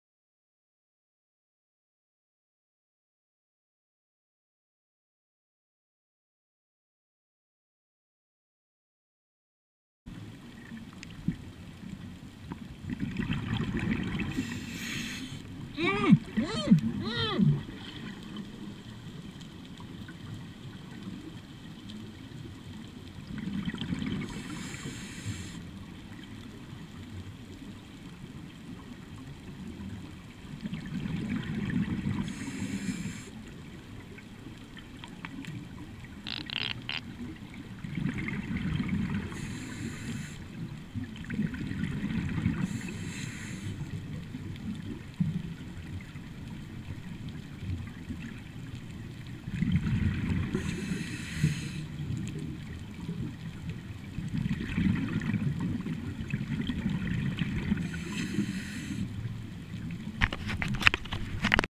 Bruce The Shark Ceti Bay Guam 28FEB16
The loud grunts are an attempt to get my dive-buddy's attention so he could see Bruce. unfortunately he missed him.